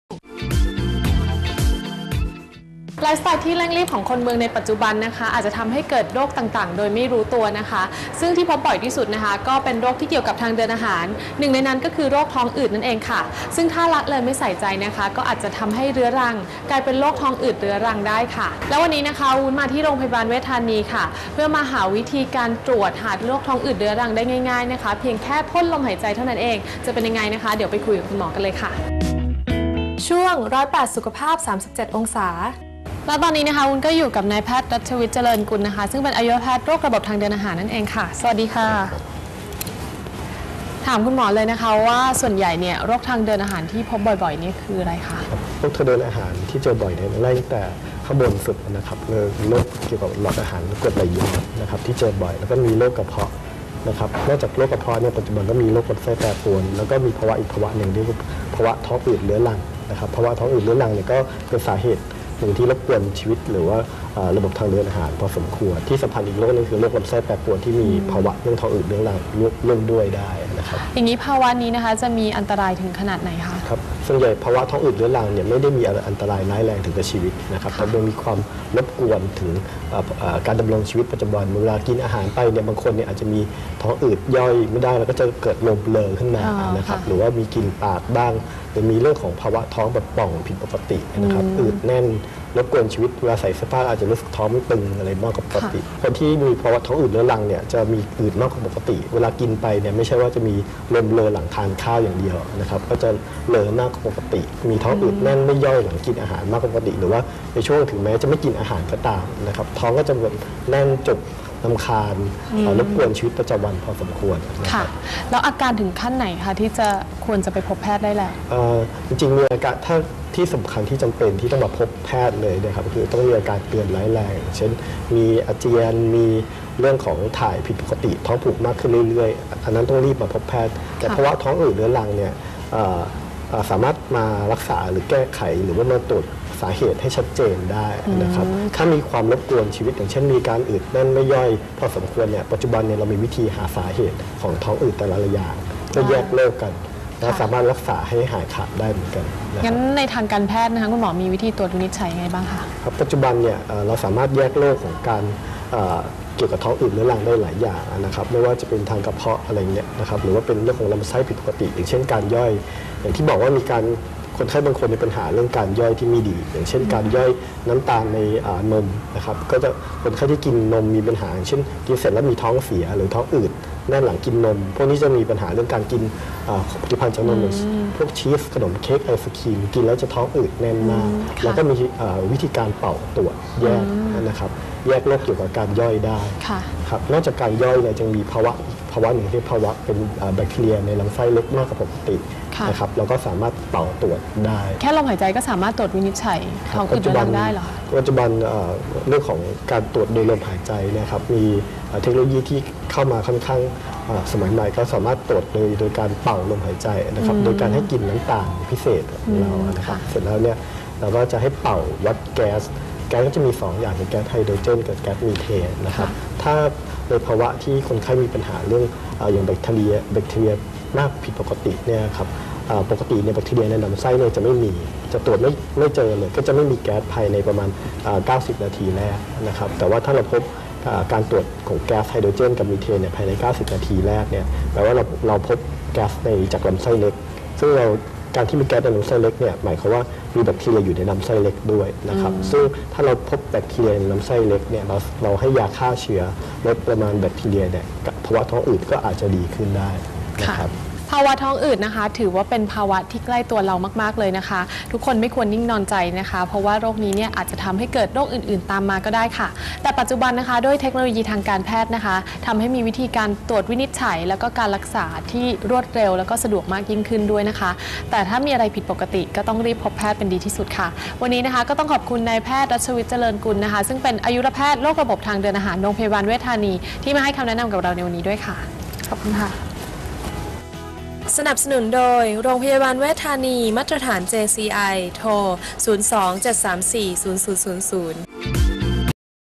พ่นลมหายใจ ตรวจภาวะท้องอืดเรื้อรัง | นพ.รัชวิชญ์ เจริญกุล
พ่นลมหายใจ ตรวจภาวะท้องอืดเรื้อรัง นพ.รัชวิชญ์ เจริญกุล อายุรแพทย์โรคระบบทางเดินอาหาร  6 เม.ษ.57 | โรงพยาบาลเวชธานี

► ติดต่อเพื่อสอบถามข้อมูลเพิ่มเติม ได้ที่